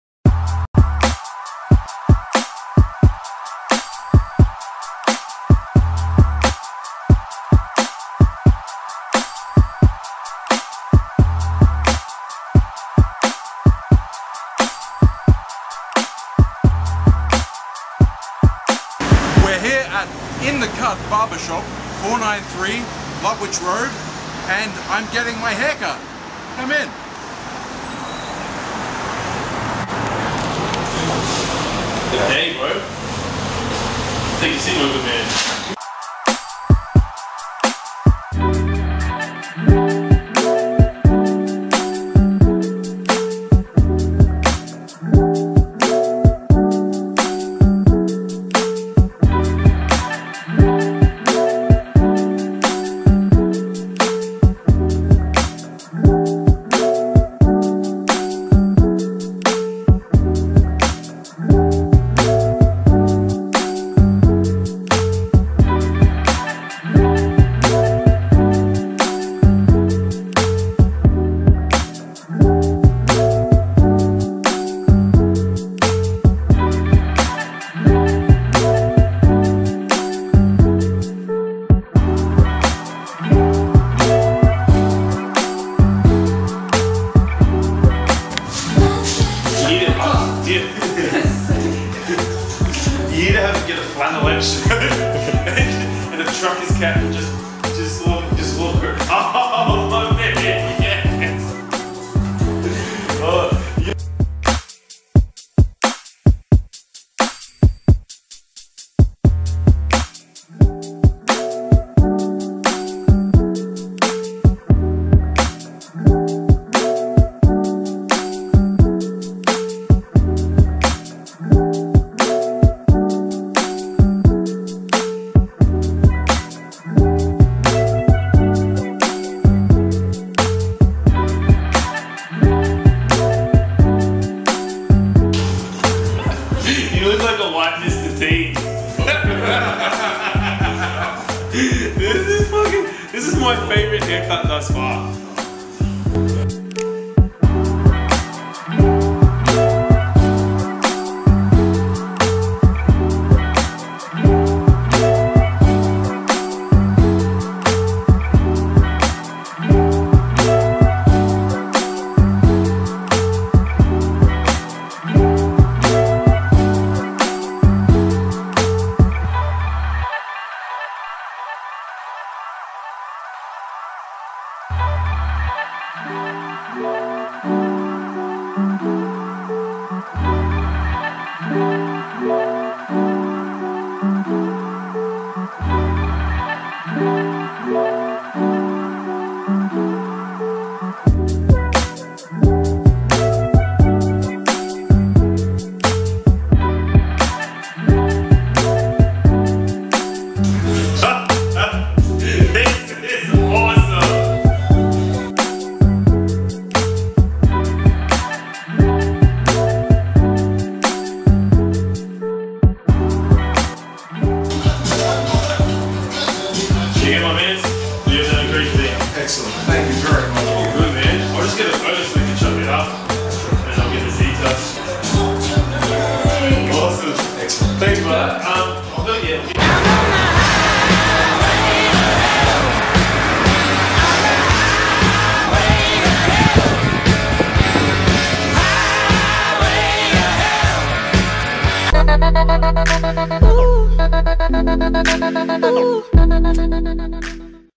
World's Greatest Shave 2017 - Matt's Haircut
My friend Matt is taking part in the Leukaemia Foundation’s World’s Greatest Shave!

He's already SMASHED his goal of $1,000 and is well on his way to $2,000
Instead of shaving all of his hair off, he put the challenge to his donors:

"For every dollar donated up to $500 I will donate 50c of my own money.
"$1000 - I will get a mullet and wear it for a week.
-- STRETCH GOAL --
$1500 - I will shave stripes into the side of my mullet." – COMPLETED

Thanks to Tevita Hala'ufia at In The Cut Barber Shop, Lutwyche